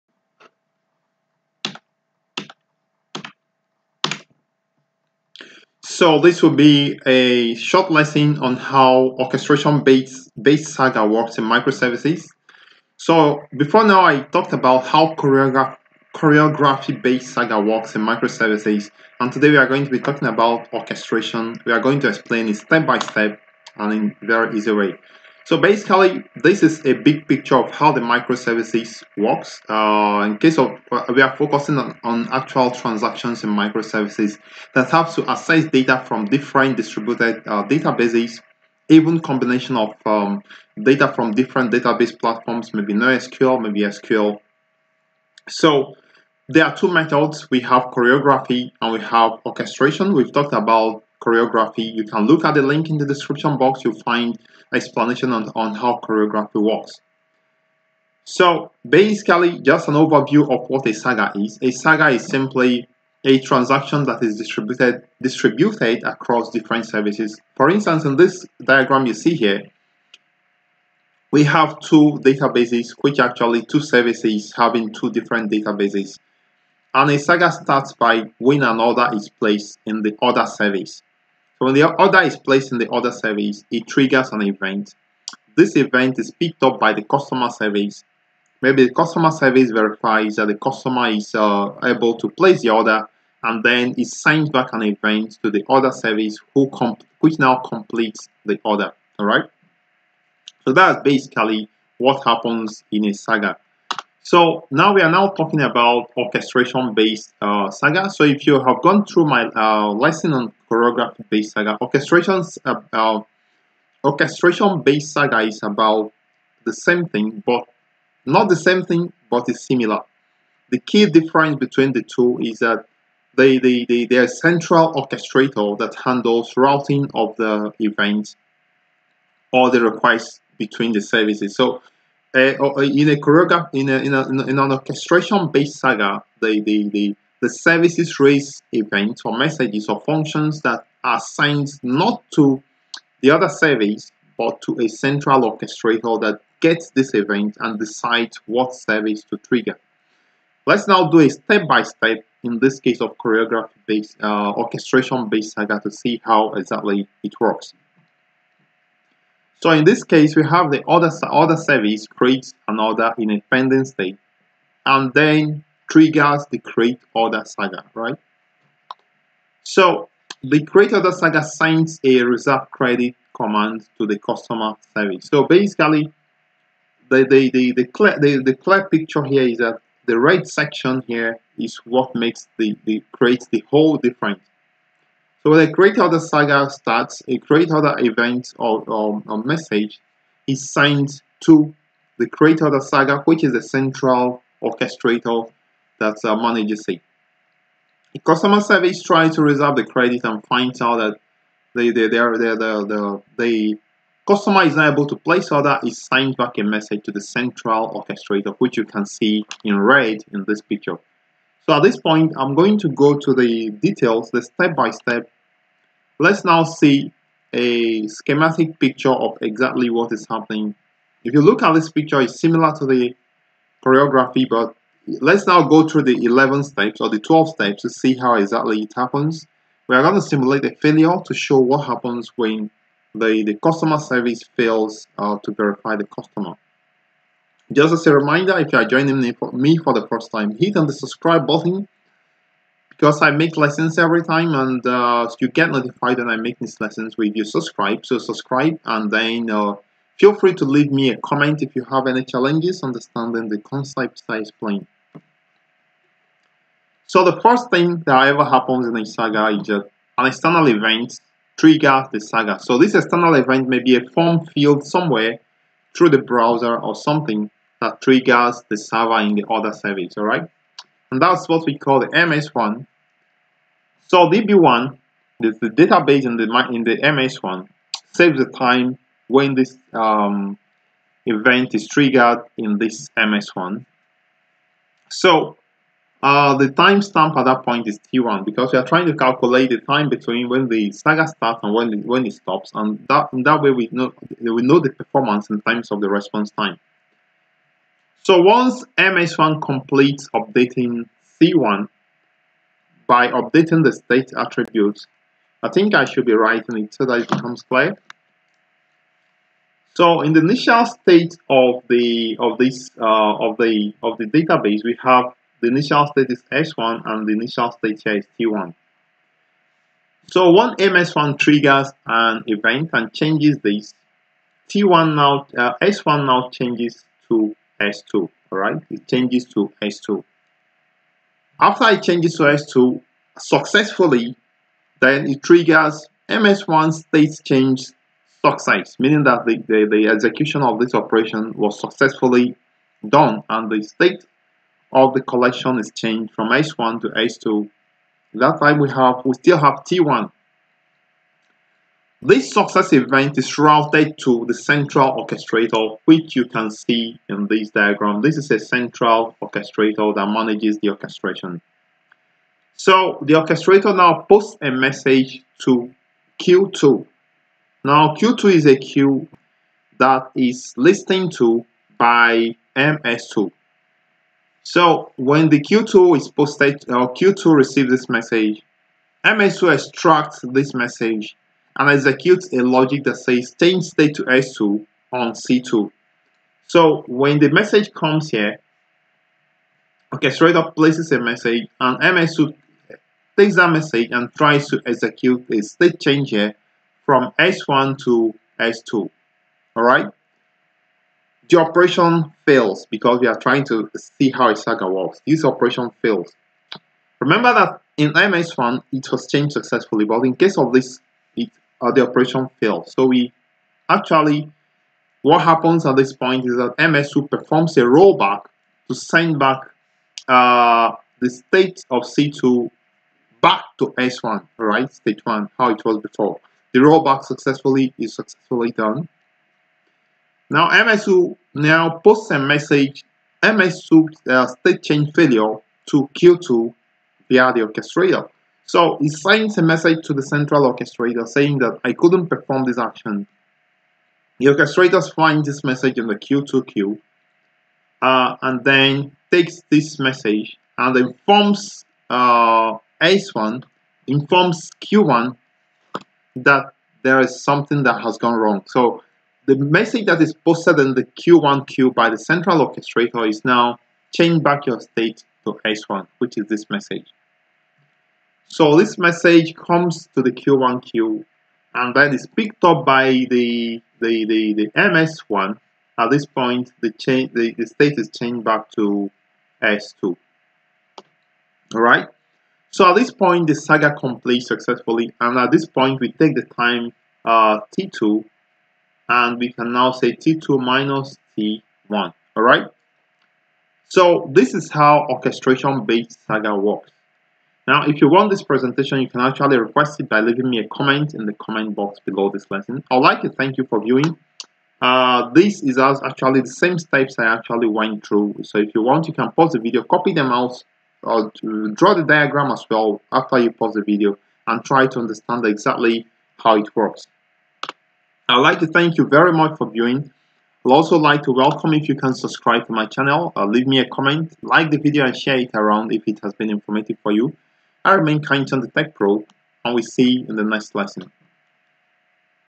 How Orchestration-Based Saga Works in MIcroservices
This video explains how Choreography Based Saga works in Microservices

How to Build Microservices in Java Using Spring Tool Suite and Eureka